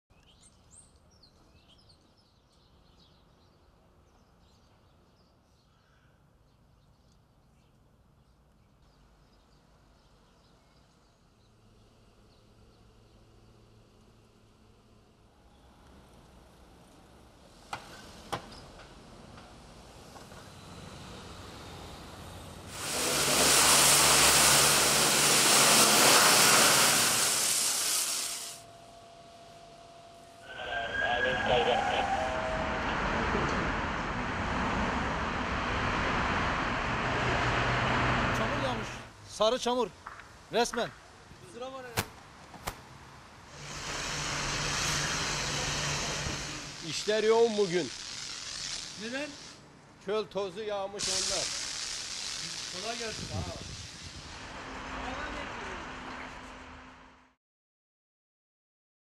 İstanbul'da Yağmurla Birlikte Çamur Yağdı!
İstanbul'da toz taşınımı nedeniyle yağmurla birlikte çamur yağdı. Sabah uyandıklarında araçlarını çamur kaplı gören vatandaşlar şaşkınlık yaşarken oto yıkamacılar yaşanan yoğunluktan memnun kaldı.